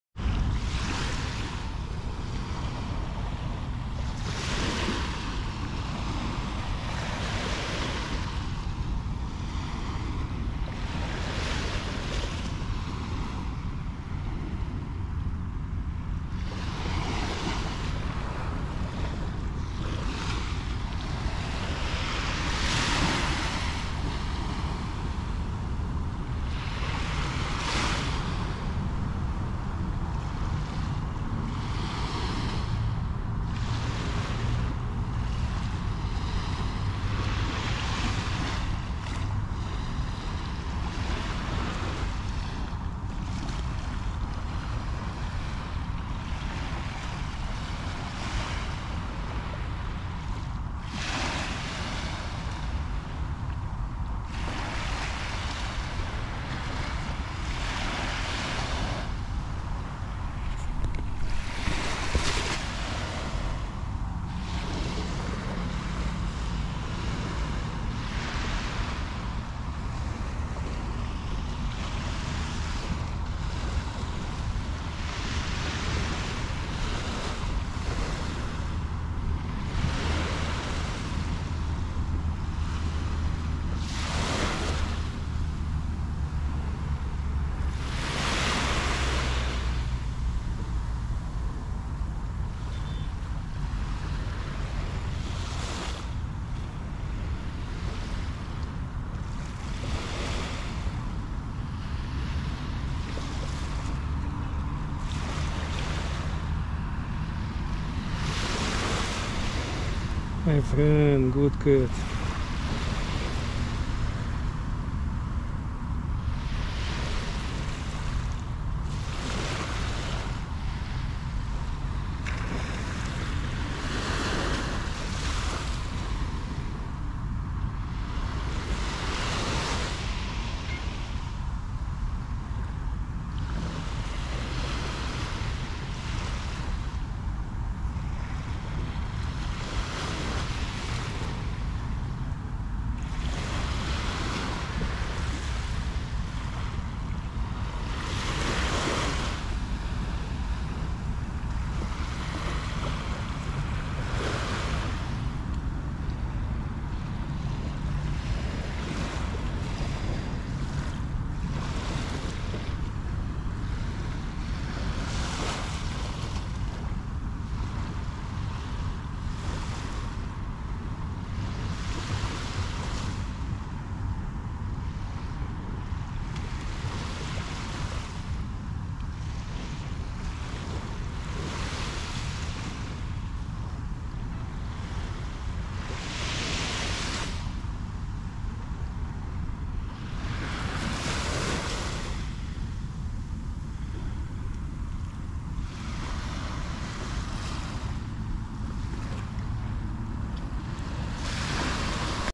INCREDIBLE!😮I was surprised when I saw how this BEACH CAT behaved🐱
I was relaxing on the beach and suddenly I saw a beautiful, beach, tabby cat approaching me. I immediately told him “welcome” and began to caress this affectionate cat. This friendly cat agreed to my caresses, moreover, he climbed onto my lap and just started looking at the waves expecting that I would caress him at the same time :). Then this cute cat tried to climb on my neck. This interesting cat wanted to be everywhere on my body :). This cat acted as if he were a house cat and not a stray cat. This is the first time I have met such tame street cats. These magnificent cats are sometimes found in Turkey.